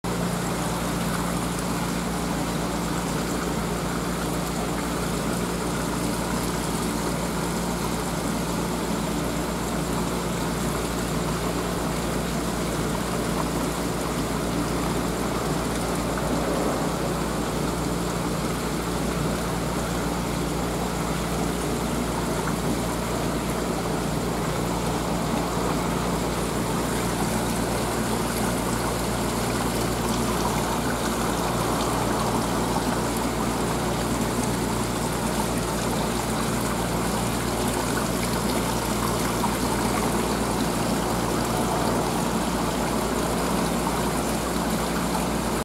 シロギスには毎日最低４回エサを与えています
スマートな体系の為？一度にあまり食べないキス（シロギス）。
1日に何度もエサを与えて長生きさせています。
水槽の中には現在5匹、元気に泳いでいます。

またファミリーや初心者さんでも安心して使用できる簡単な仕掛けや竿、釣り場案内や仕掛け図等をご用意してお待ちしております。